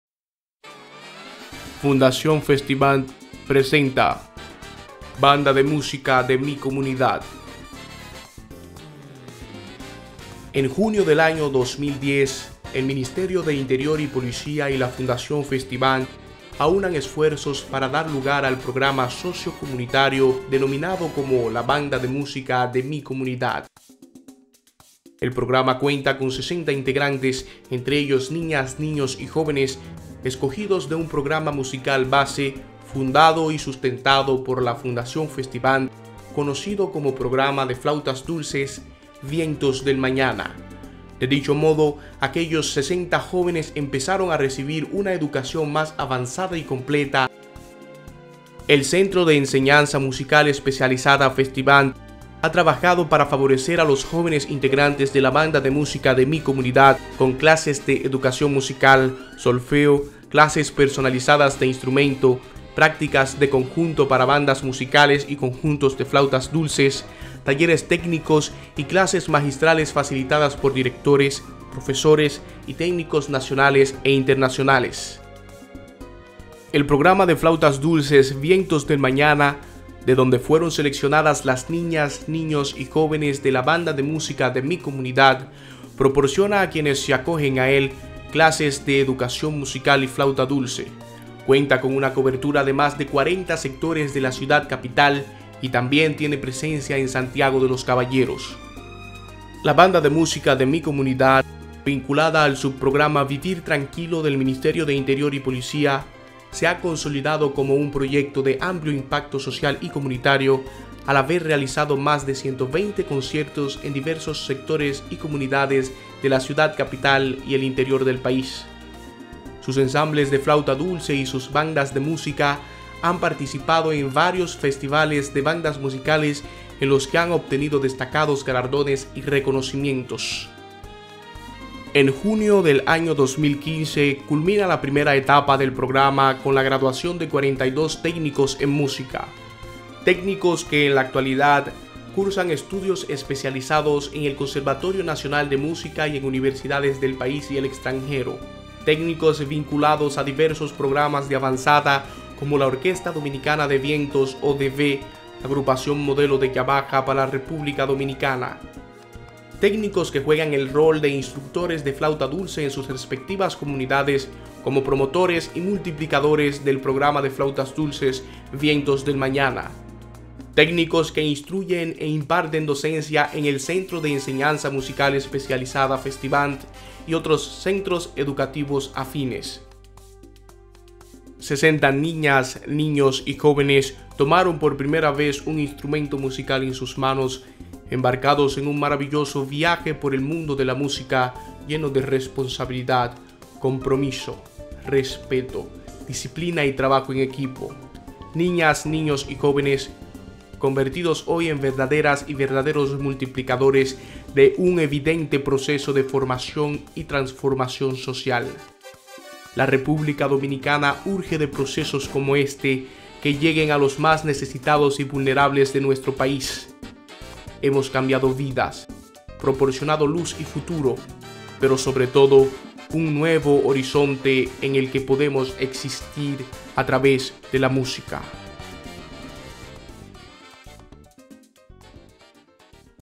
Programa Banda de Música Mi Comunidad
Este programa de la banda de música Mi Comunidad auspiciado por Vivir Tranquilo del Ministerio de Interior y Policía y sustentado por el centro de enseñanza musical Festi-Band, inicia en junio del 2010.

"Hemos cambiado vidas, proporcionado luz y futuro, pero sobre todo un nuevo horizonte en que podemos  existir a través de la música".